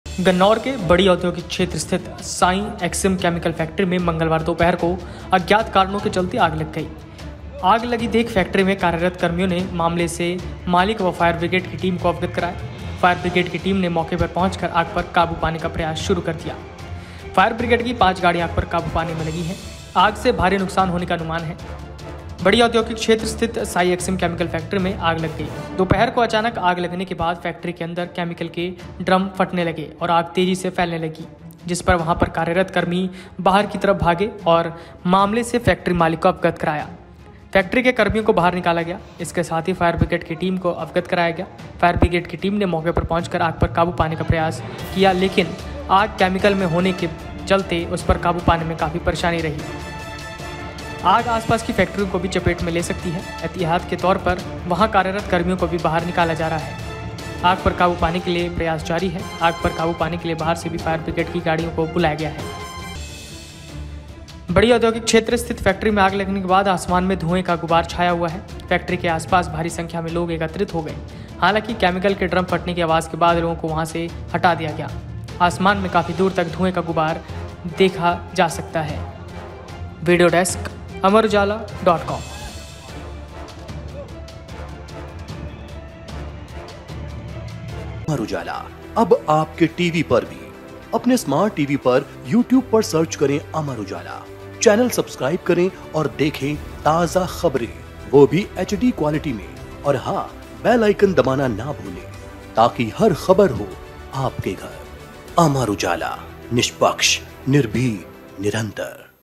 Sonipat Fire: सोनीपत के बड़ी औद्योगिक क्षेत्र में भीषण आग लगने से लोगों में मचा हड़कंप
गन्नौर के बड़ी औद्योगिक क्षेत्र स्थित साई एक्सिम केमिकल फैक्टरी में मंगलवार दोपहर को अज्ञात कारणों के चलते आग लग गई। आग लगी देख फैक्टरी में कार्यरत कर्मियों ने मामले से मालिक व फायर ब्रिगेड की टीम को अवगत कराया।